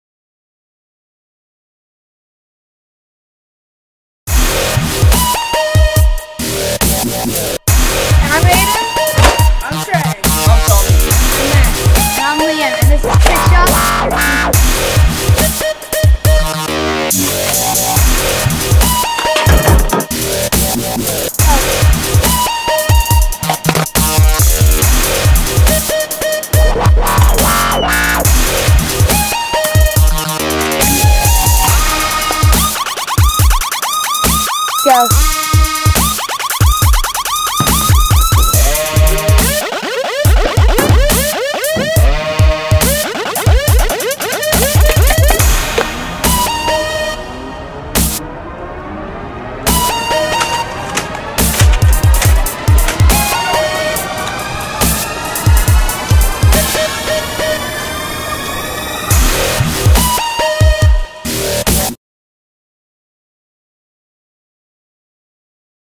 Trick Shots 4 /w Aiden, Tre, Tommy and Max
This is the fourth version of trick shots.
Filmed with Canon HD Video and GoPro Hero3+

Tre-Sean Green
Aiden Wilson
Tommy Hosty
Liam Hosty
Max Merritt